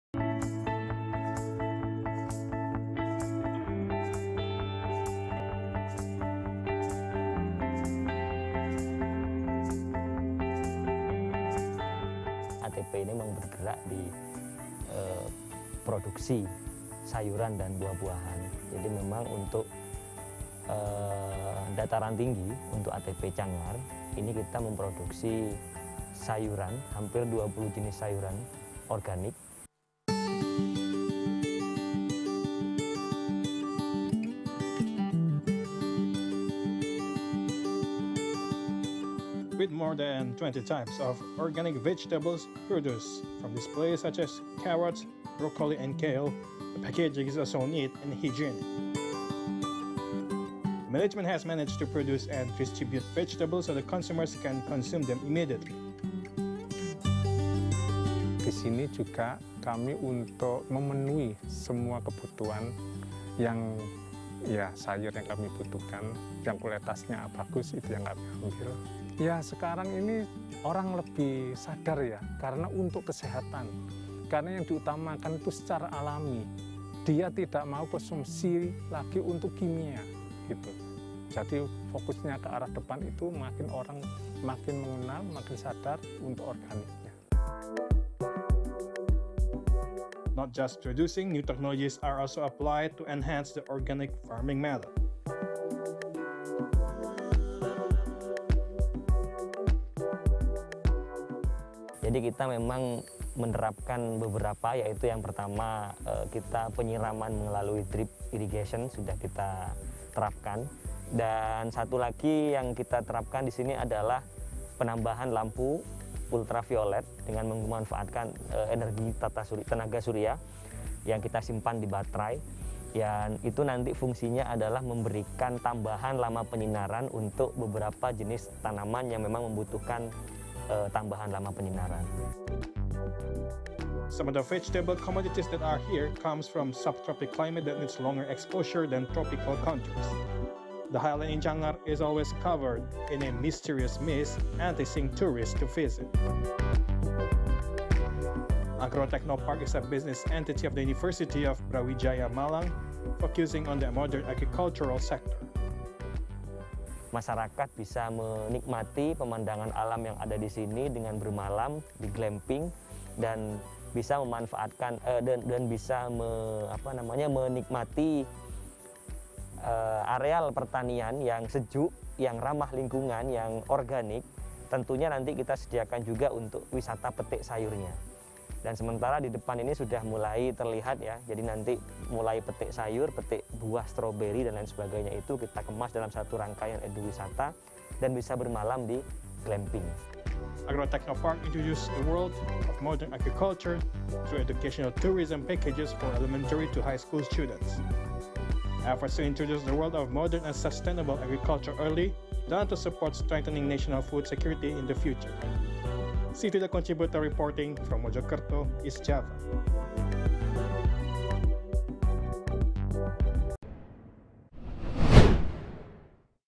Agro Techno Park
Agro Techno Park, dataran dengan ketinggian 1700 mdpl, di Desa Sumberbrantas, Kota Batu Awa Timur.
Area pertanian seluas 14 hektar ini menggabungkan unsur penelitian, inovasi kreatif, serta memberikan edukasi seputar pertanian modern kepada para petani.
Agro Techno Park, a plain with an altitude of 1700 meters above sea level, in Sumberbrantas Village, Batu Awa City Timur.
This 14-hectare agricultural area combines elements of research, creative innovation, and provides education about modern agriculture to farmers.